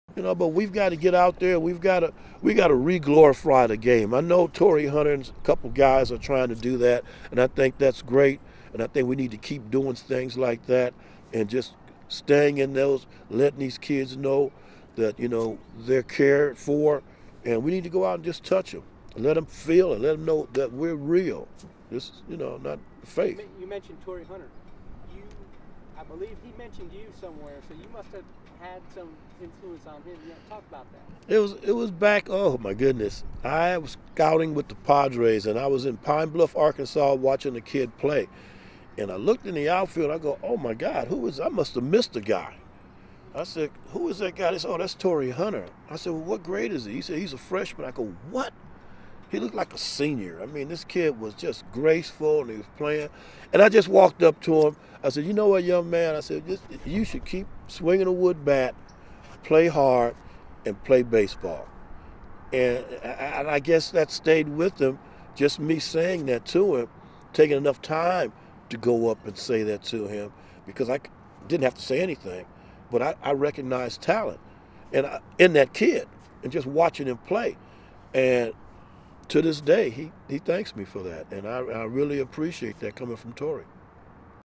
Scipio Spinks on Torii Hunter
Former MLB pitcher, now Astros scout, Scipio Spinks describes the first time he saw Torii Hunter as a freshman in high school.

This was part of an interview recorded for a documentary film about the late Dock Ellis, a friend of Scipio.  Filmed at Dell Diamond, home of the Round Rock Express.